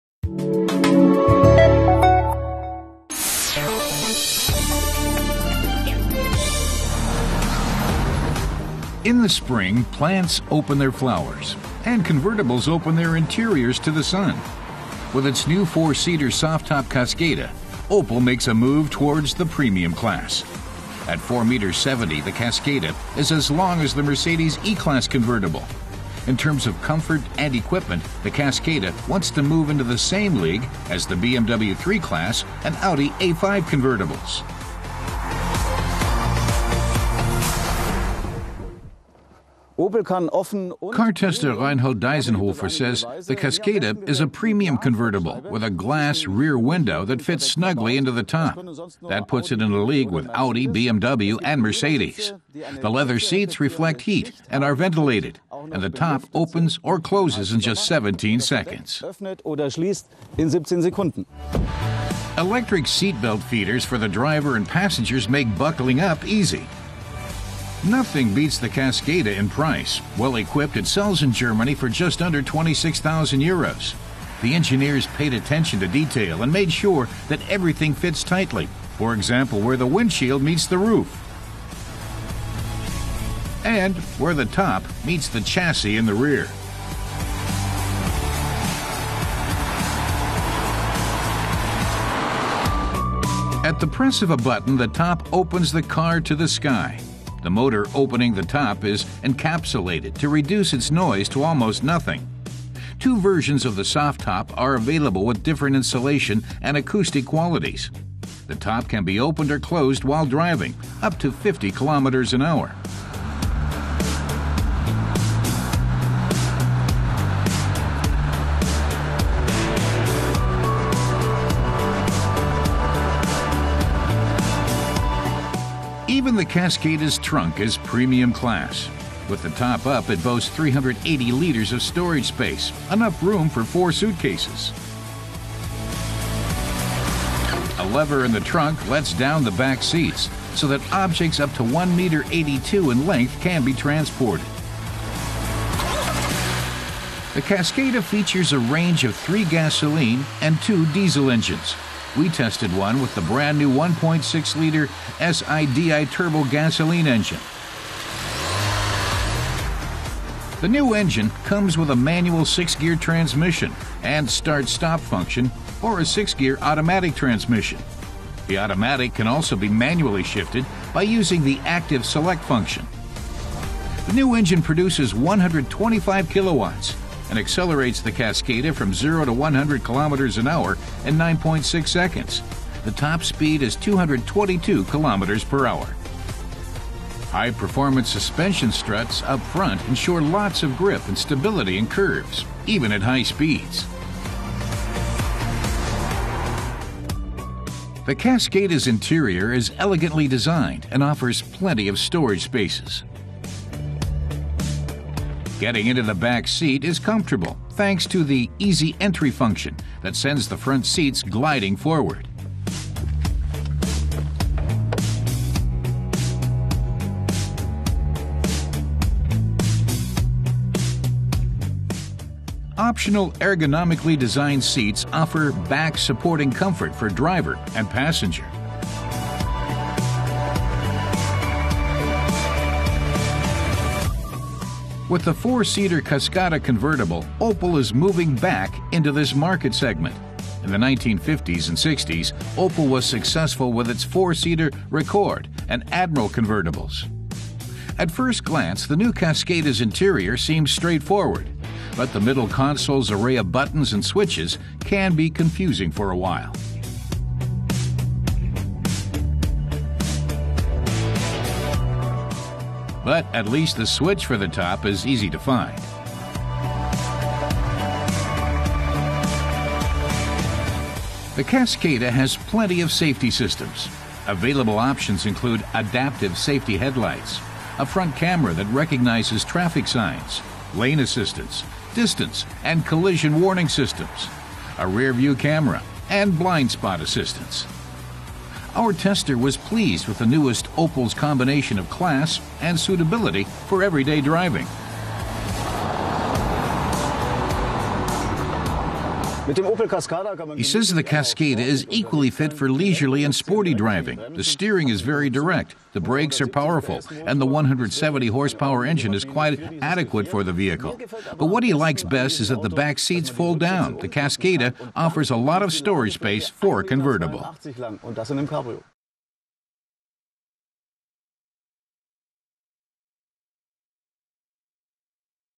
New convertible - Opel Cascada | Drive it!
Opel's new four-seater soft-top Cascada convertible is making a bid to enter the premium class. At 4.7 meters long, Opel is targeting the premium category, including the BMW 3 series and the Audi A5.